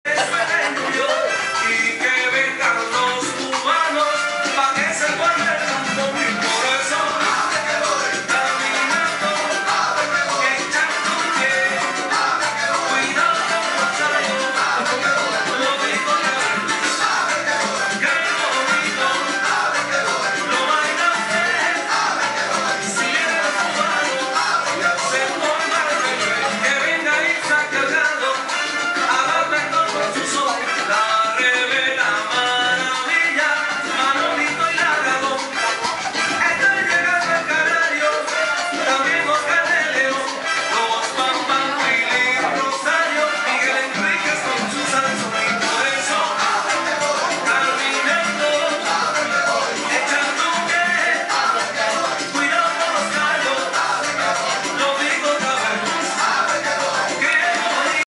Ed and Venita (Wed, Sept 16th)
Dancing at Sultry Swing.  West Coast Swing, Hustle, Salsa, Ballroom, and Country!